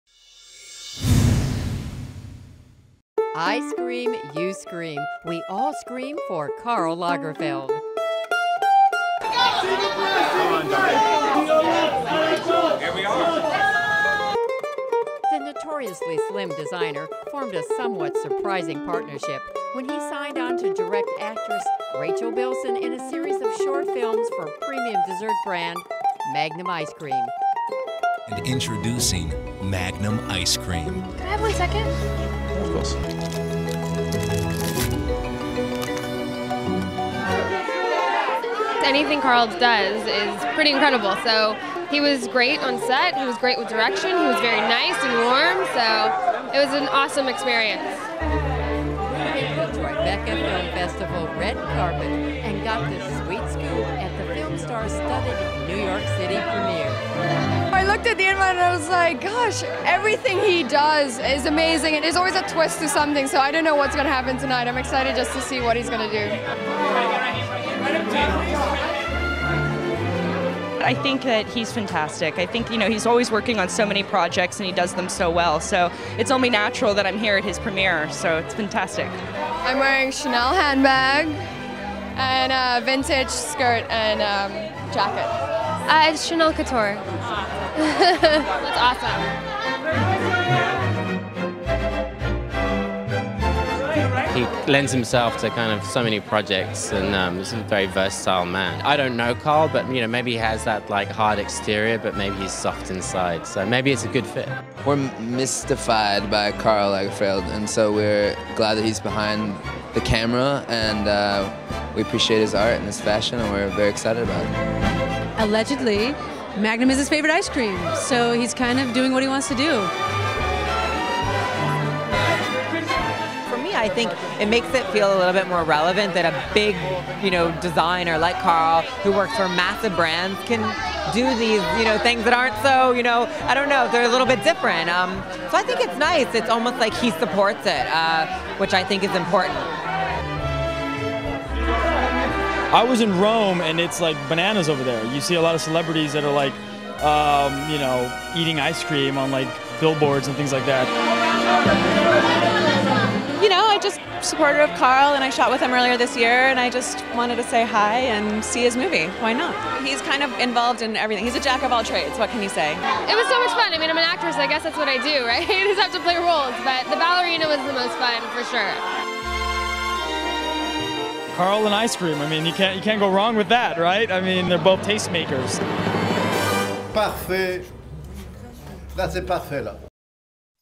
Magnum at the Movies | VF STYLE
Karl Lagerfeld directs deliciously short films for magnum ice cream, starring actress Rachel Bilson.